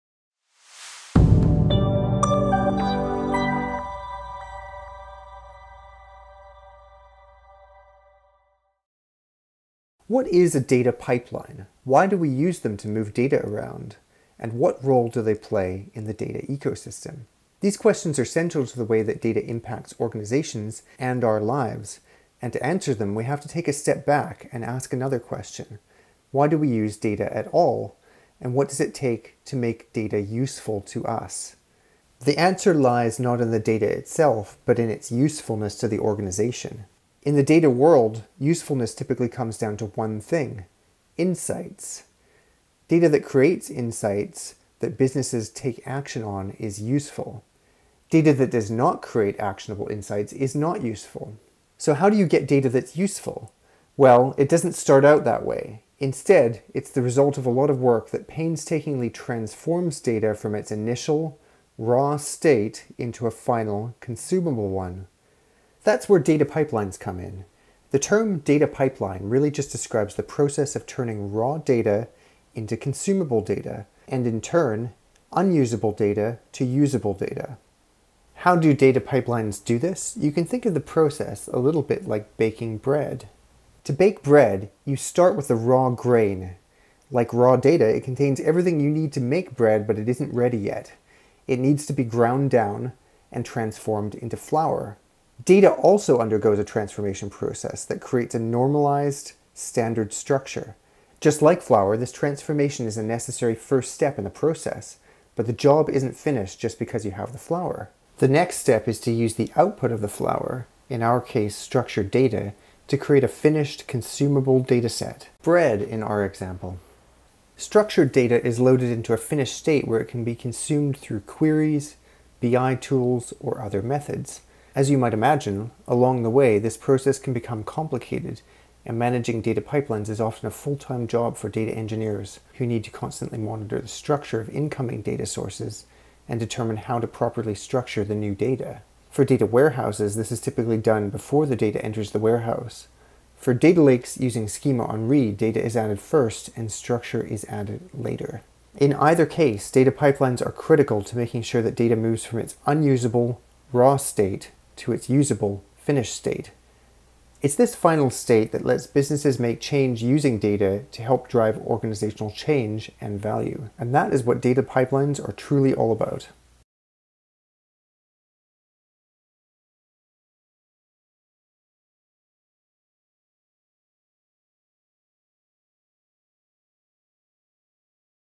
What is a data pipeline? | Starburst Academy
Ever wanted to know more about data engineering? This video tutorial explores the purpose and value of data pipelines and data engineering in more depth, including their use in modern data lakes and data lakehouses. It also outlines the topics covered in the Starburst Academy course, Exploring data pipelines.

Lessons include:
-What is a data pipeline?
-ETL and data pipelines
-Data pipelines and data lakes
-Starburst and data pipelines
-Starburst and dbt
-Starburst Galaxy step-by-step tutorial

For more information on this topic check out the full course, Exploring data pipelines It digs deeper into data pipelines at all levels, including a discussion of how Starburst Galaxy can help construct a pipeline using SQL. It is available on Starburst Academy

The course is part of the Data foundations series The series provides a conceptual and contextual background to help you understand how Starburst fits within Big Data ecosystems, including Exploring data lakes and Exploring data lakehouses Each course in the series is suitable for all audiences.